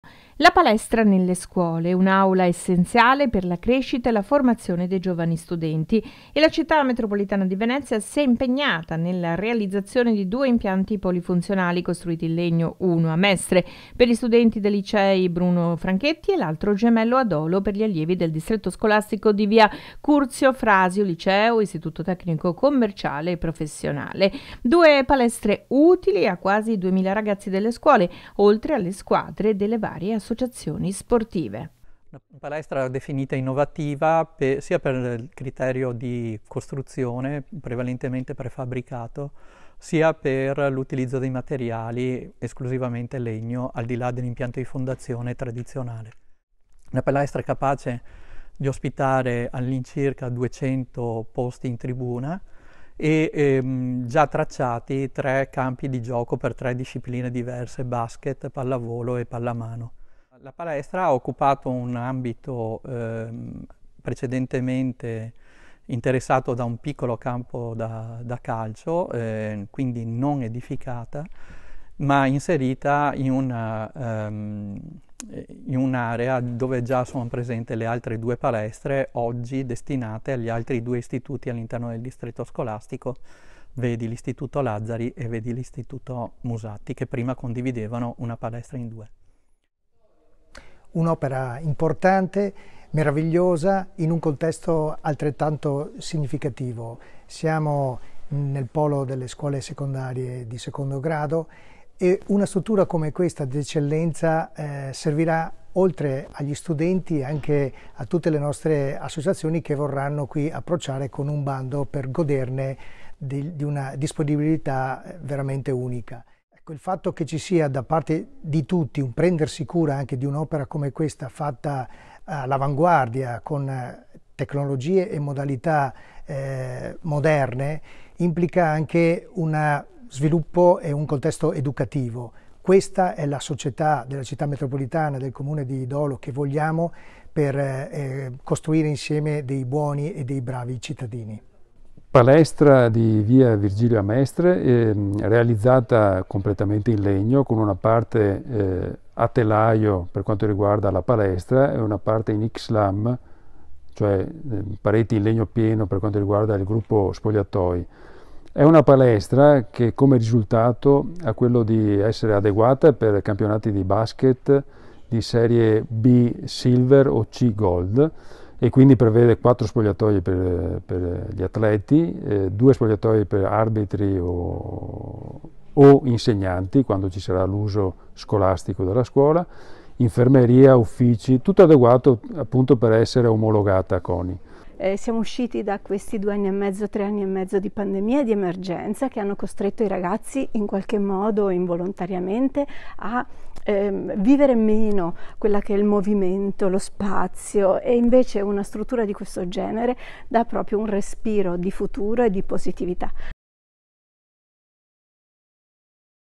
LE PALESTRE GEMELLE
VENEZIA : una buona fetta di fondi del Pnrr saranno impiegati per l’edilizia scolastica. La città metropolitana di Venezia ha già realizzato due palestre nuove di zecca per gli istituti di Mestre e Dolo.